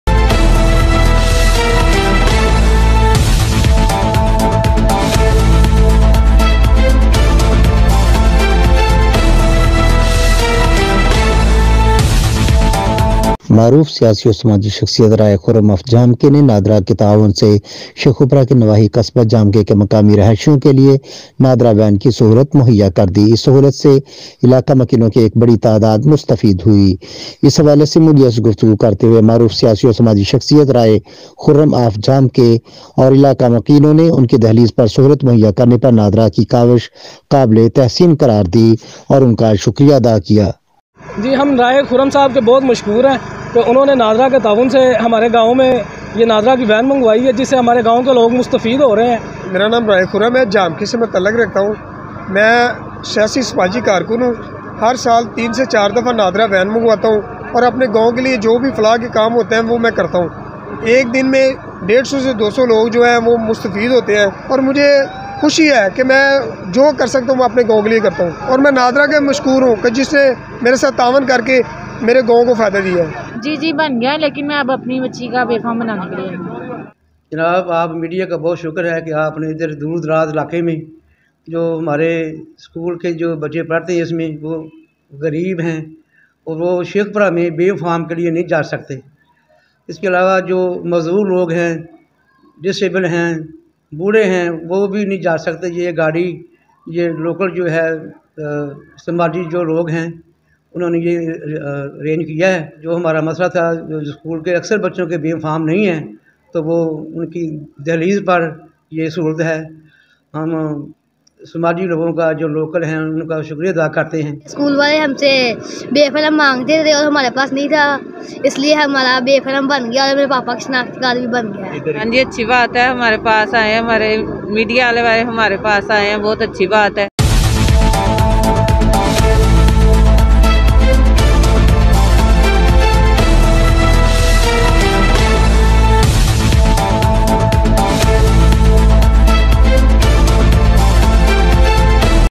معروف سیاسی و سماجی شخصیت رائے خرم آف جھامکے نے نادرا کے تعاون سے نادرا وین کی سہولت مہیا کردی
معروف سیاسی و سماجی شخصیت رائے خرم آف جھامکے نے نادرا کے تعاون سے شیخوپورہ کے نواحی قصبہ جھامکے کے مقامی رہائشیوں کے لئے نادرا وین کی سہولت مہیا کردی